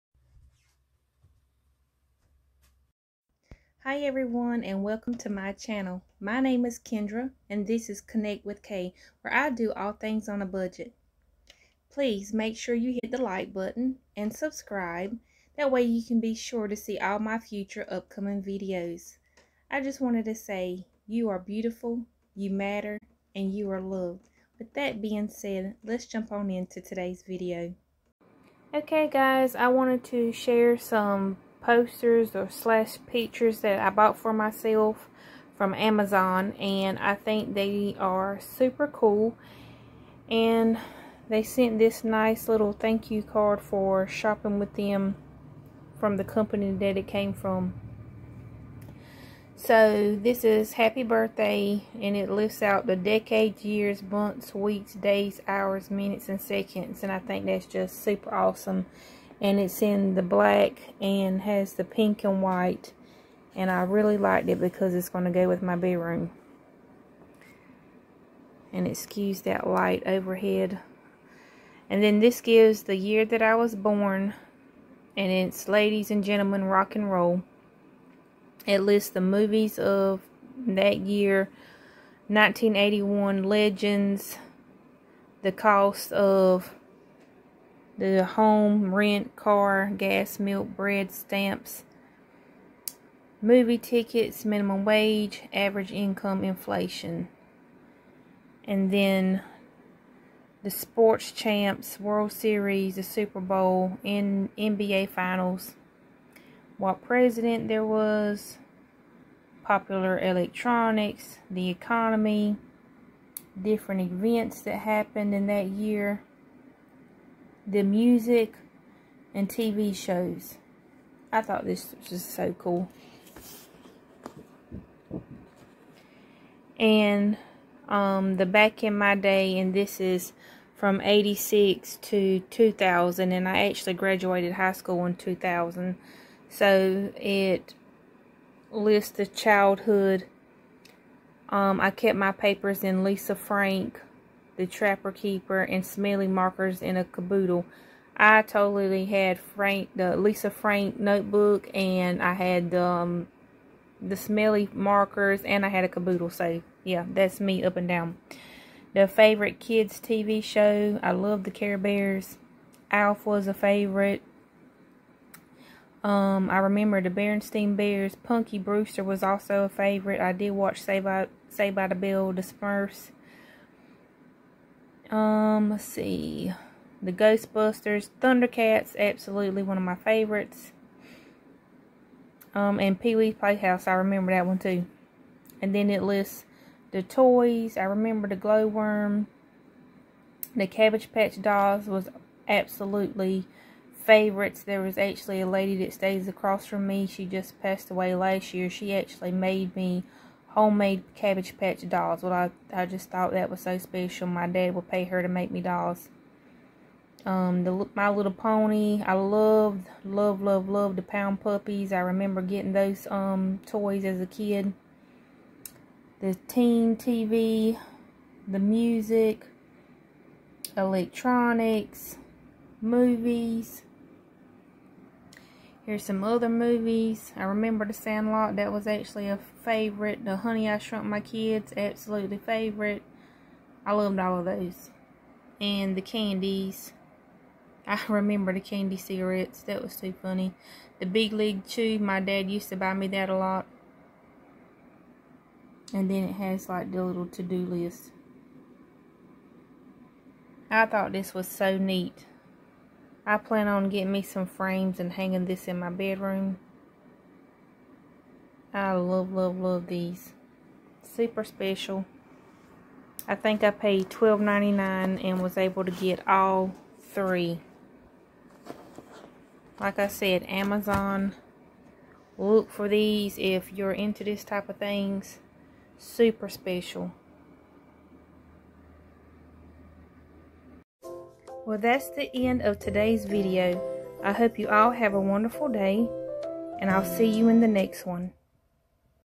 My Birthday Find😻 Amazon Find!!
This was a super special find on Amazon. I think these signs are so incredibly neat! Amazon has Everything ‼️